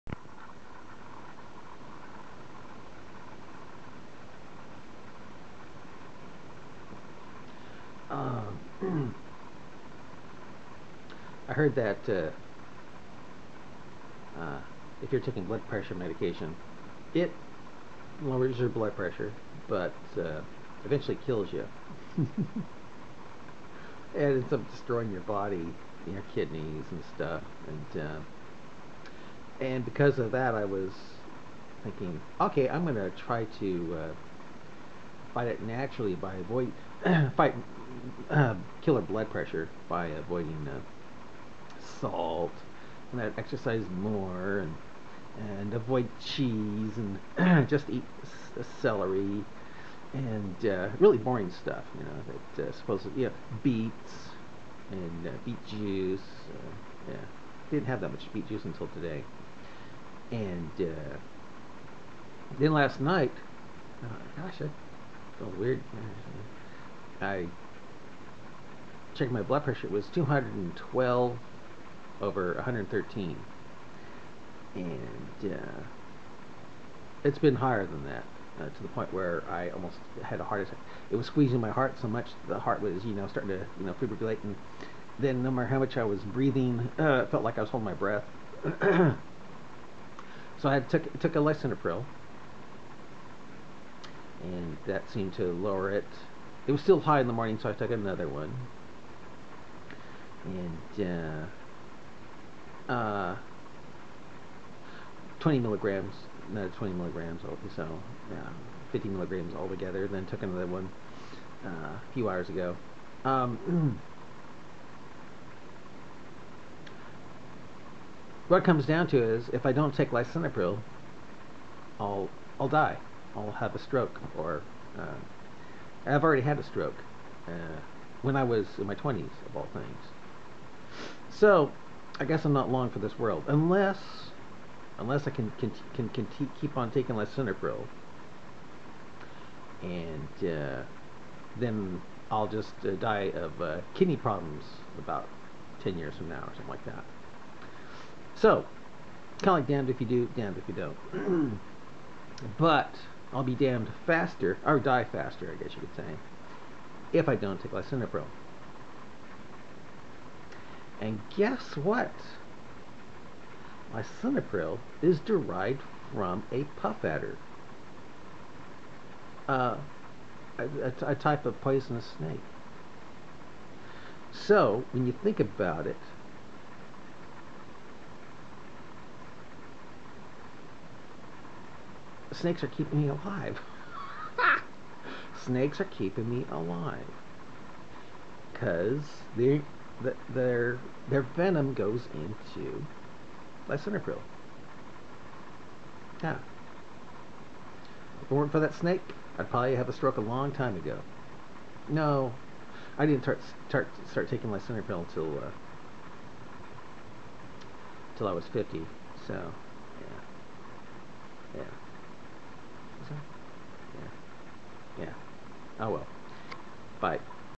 Literal Snakes Are Saving My Life
Sounds crazy, but, deadly pit viper snakes are saving my life through lisinopril.